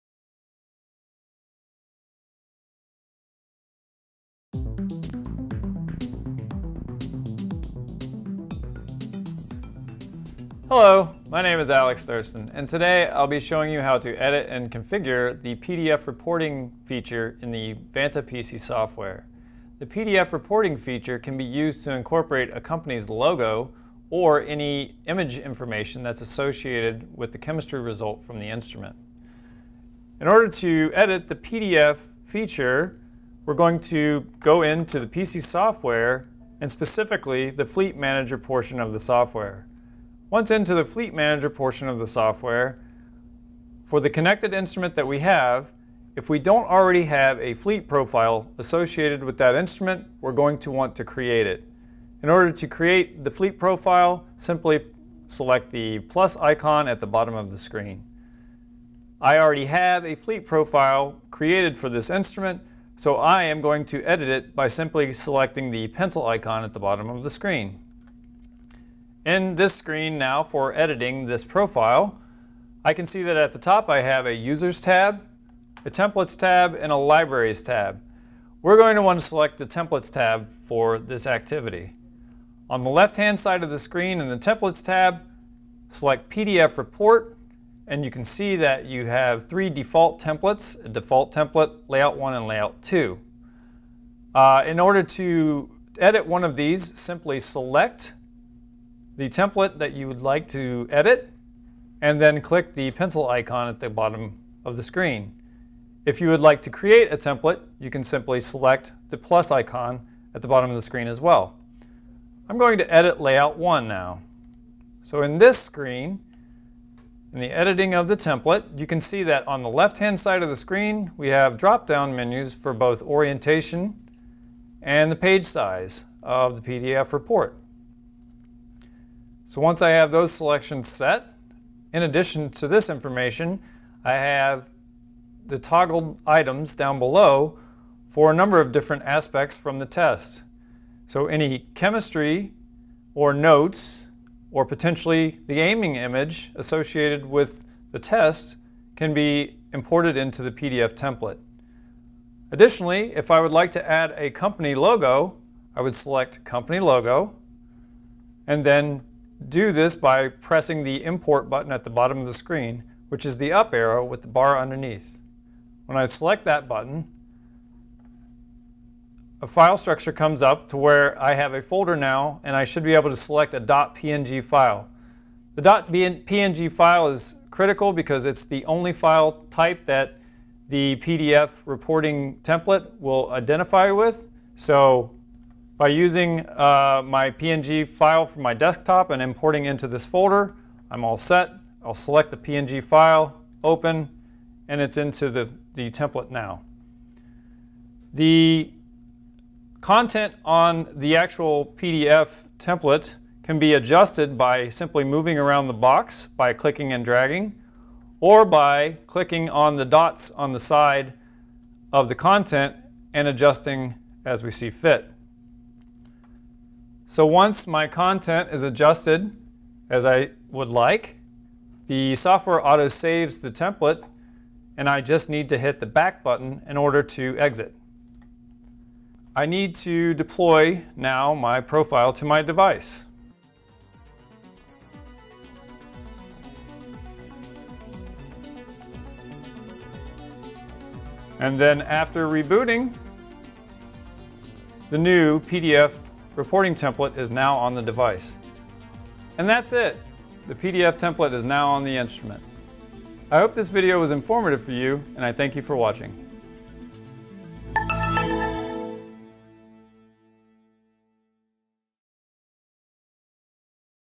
Vanta™ Technical Tutorial Series | Editing PDF Reporting Templates
This video will review how to configure and edit PDF reporting templates for the Vanta handheld XRF analyzer. By customizing the pdf form, items such as company logos or images taken during analysis can be displayed in the PDF report along with the chemistry result.